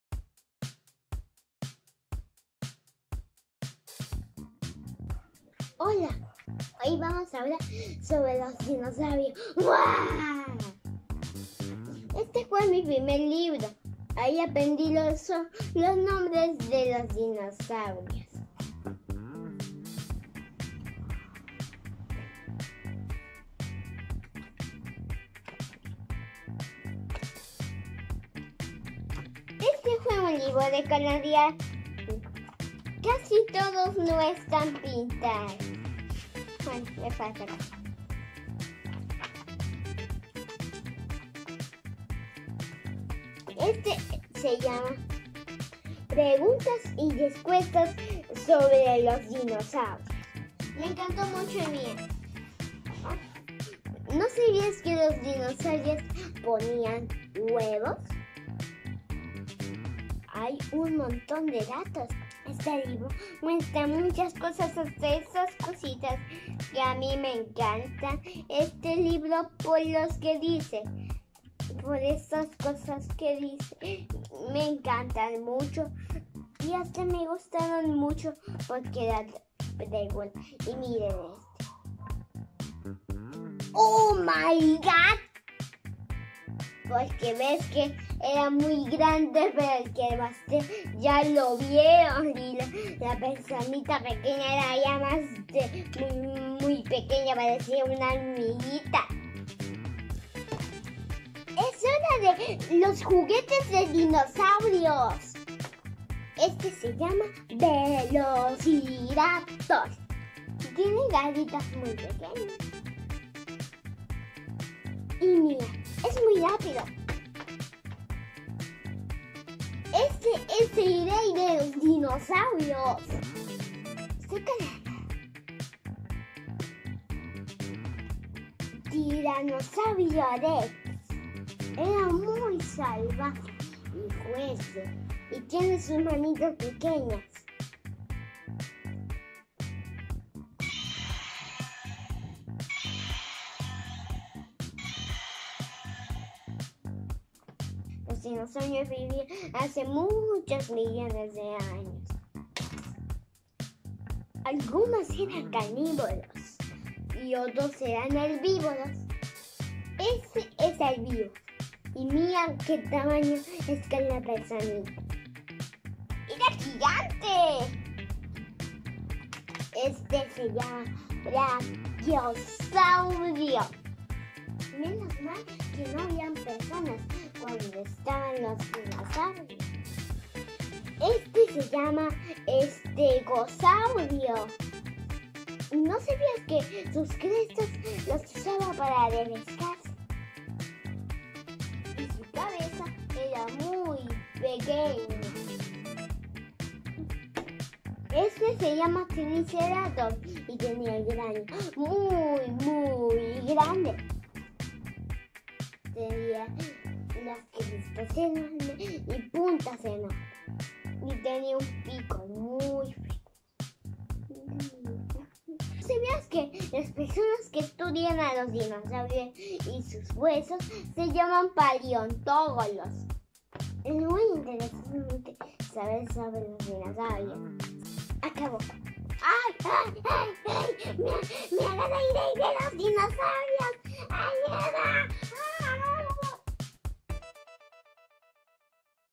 Conociendo los dinosaurios
En esta oportunidad vamos a conocer un poquito más sobre los dinosaurios.

PD: y por si acaso, el término correcto es PALEONTÓLOGOS, no PALEONTÓGOLOS.